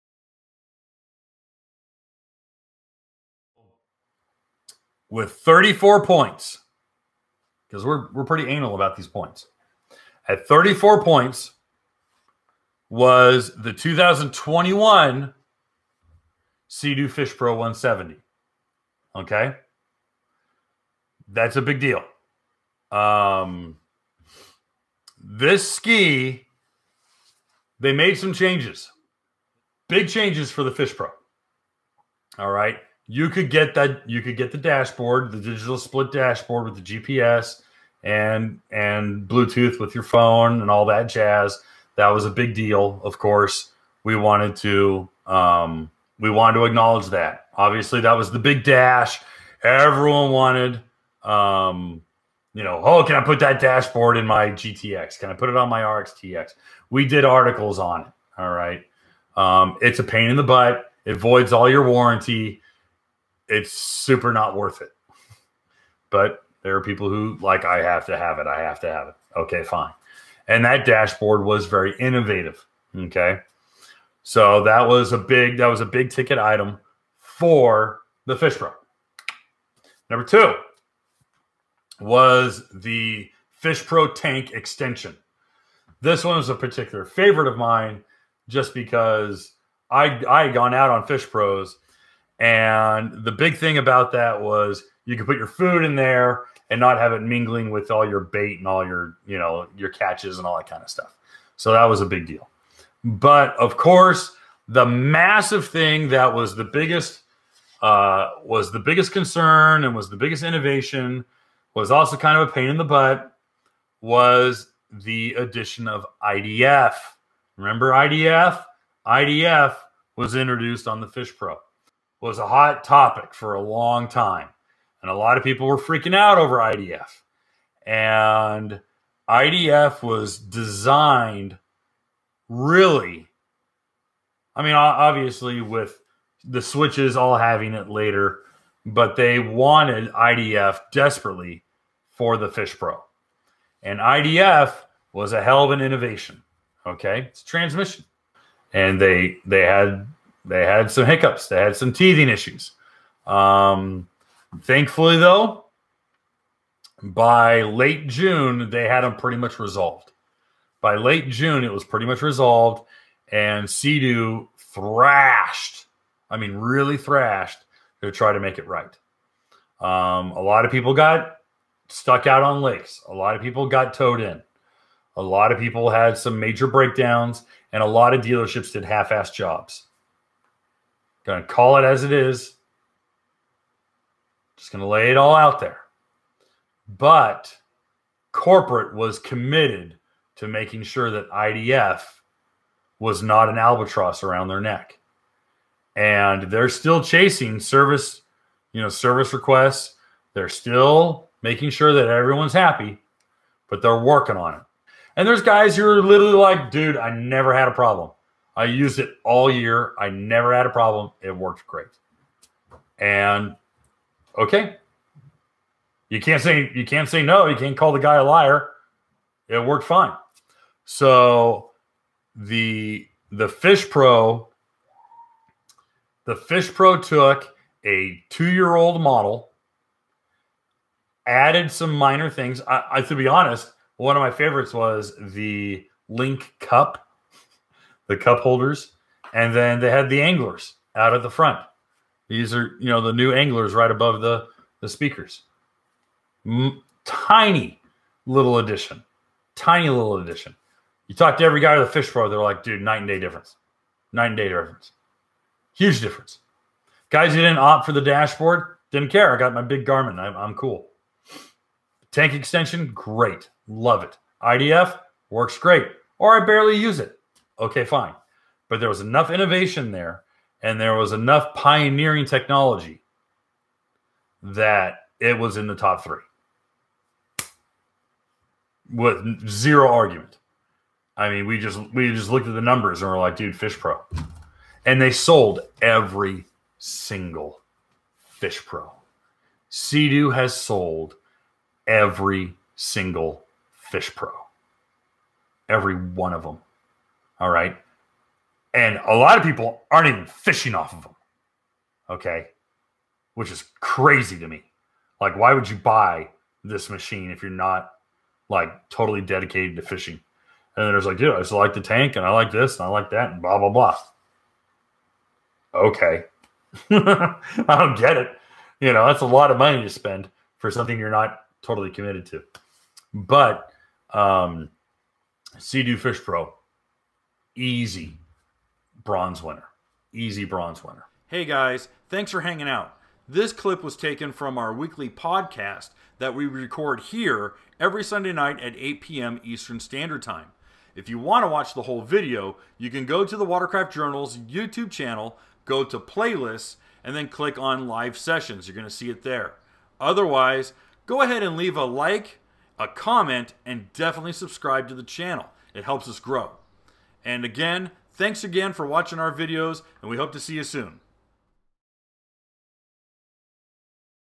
How The 2021 Sea-Doo Fish Pro 170 Won Bronze: WCJ Quick Topic
This Quick Topic segment was taken from our full-length podcast titled "Recapping 2021 Watercraft of The Year" and can be viewed in its entirety